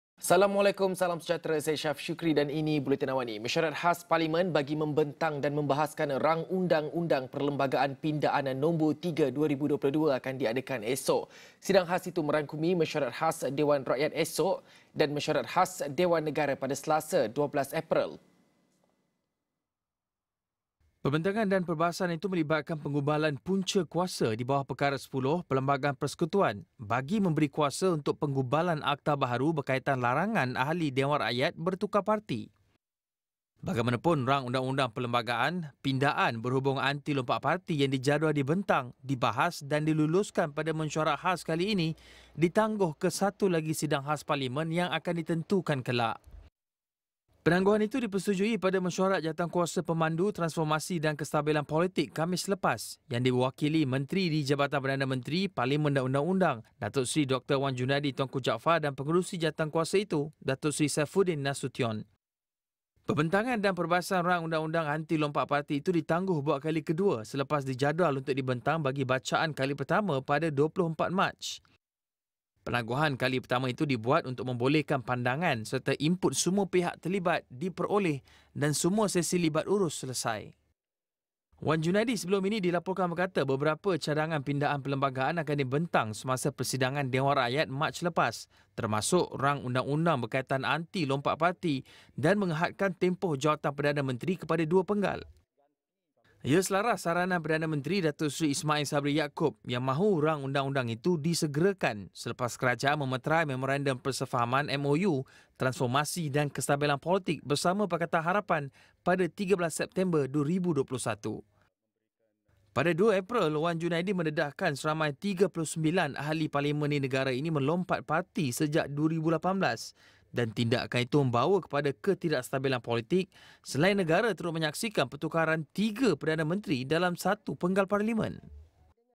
RUU Pindaan (no.3) | Sidang Khas Parlimen mulai esok
Mesyuarat Khas Parlimen bagi membentang dan membahaskan Rang Undang-Undang Perlembagaan (Pindaan) (no. 3) 2022 akan diadakan esok.

Sidang khas itu merangkumi mesyuarat khas Dewan Rakyat esok dan mesyuarat khas Dewan Negara pada Selasa (12 April).